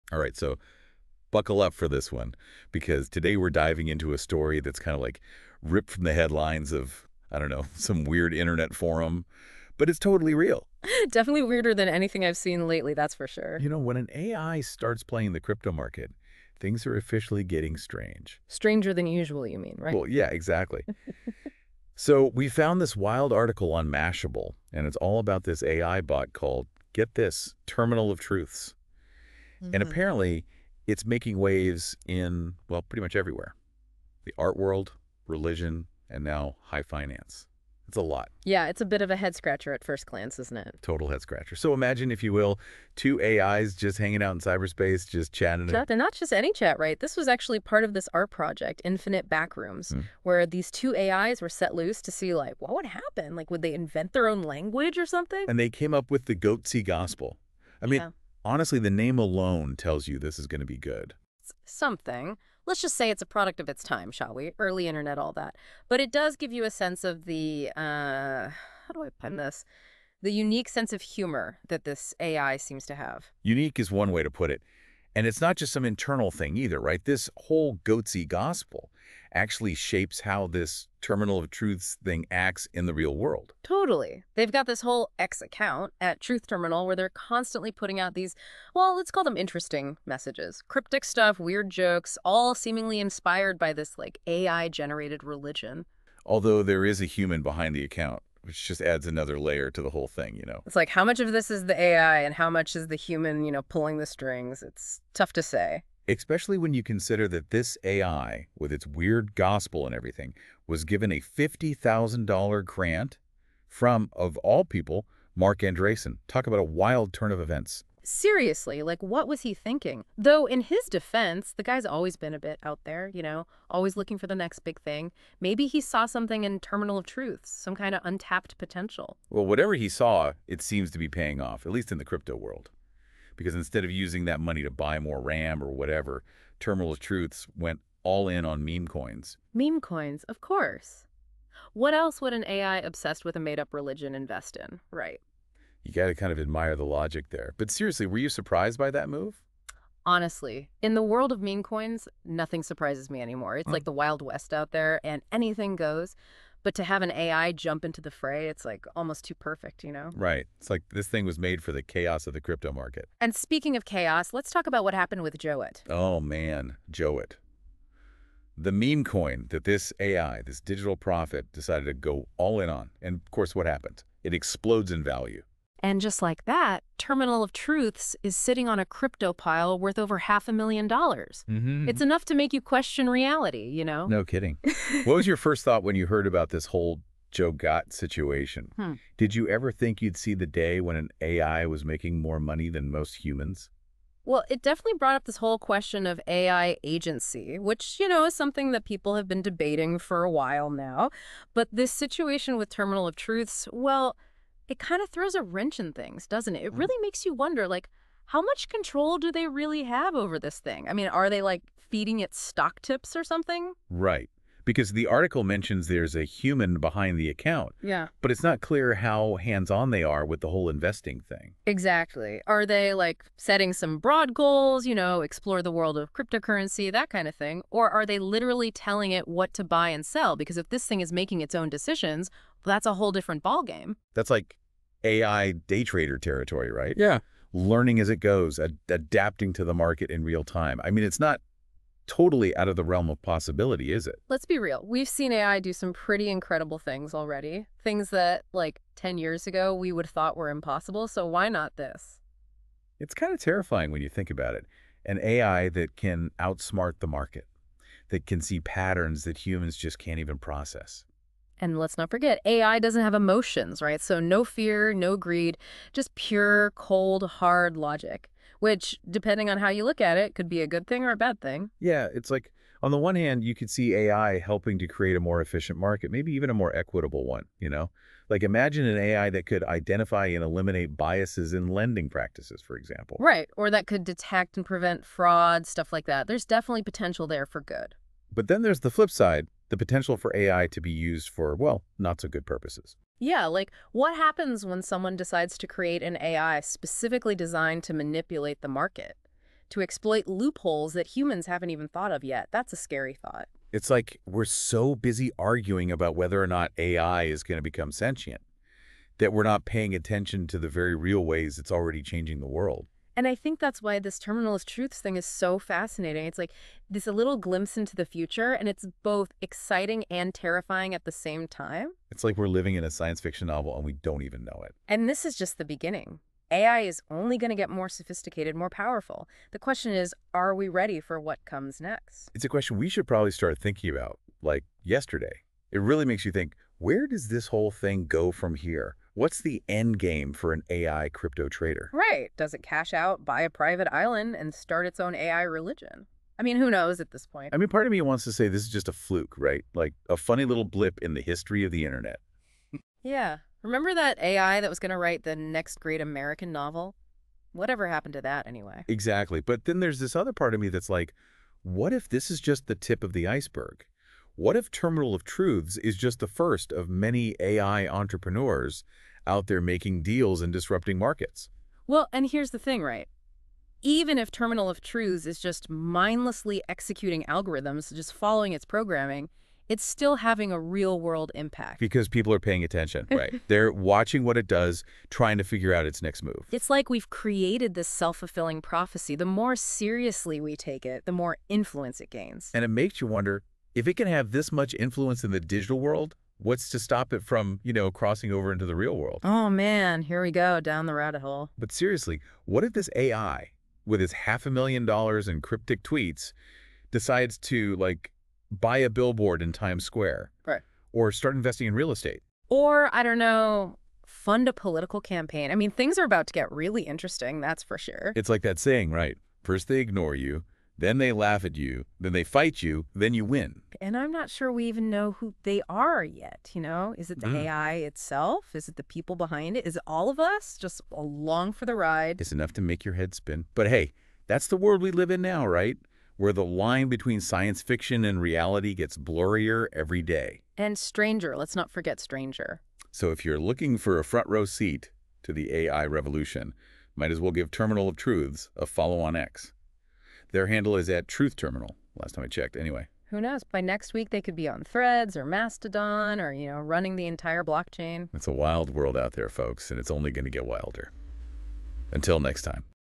How an AI became very RICH off Crypto
Here's a story of how an AI got rich off a meme-coin